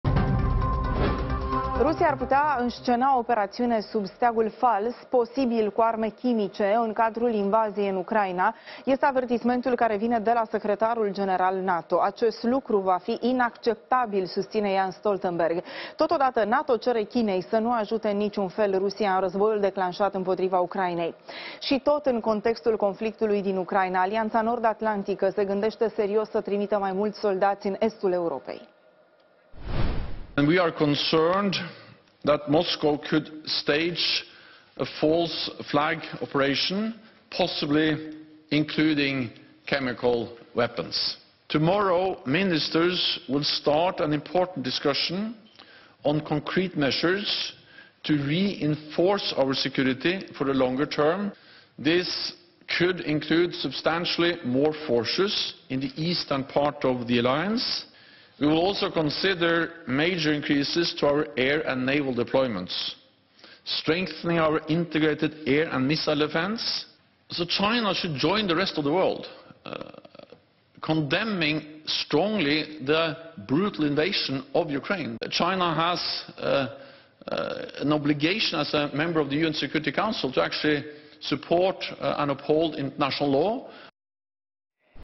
Stoltenberg: Suntem îngrijorați că Moscova ar putea înscena o operațiune sub steag fals
NATO este îngrijorată că Rusia ar putea înscena o operațiune sub steagul fals, posibil cu arme chimice, în cadrul invaziei în Ucraina, a spus marți secretarul general al NATO, Jens Stoltenberg.